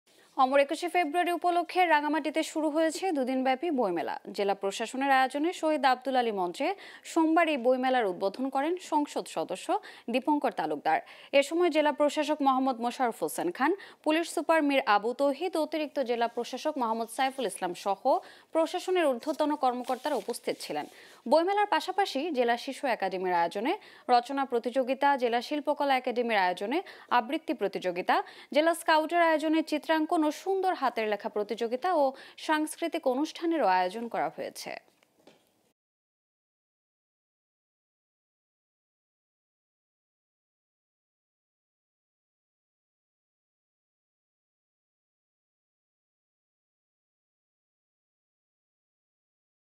২১ ফেব্রুয়ারি উপলক্ষে রাঙামাটিতে ২ দিনব্যাপী বইমেলা শুরু | Global Television
২১ ফেব্রুয়ারি উপলক্ষে রাঙামাটিতে ২ দিনব্যাপী বইমেলা শুরু | Global Television